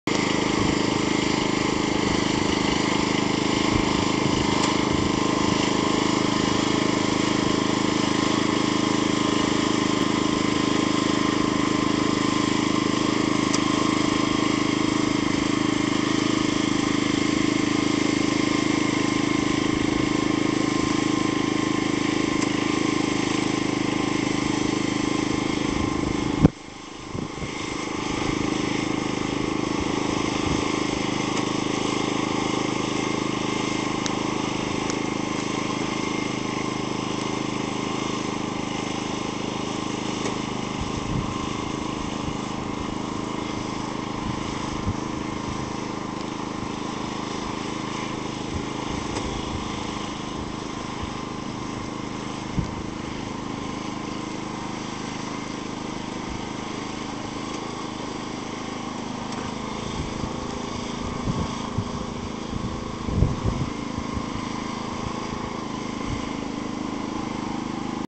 2010年6月1日田植え
毎年の事ながら今年も田植えが始まりました。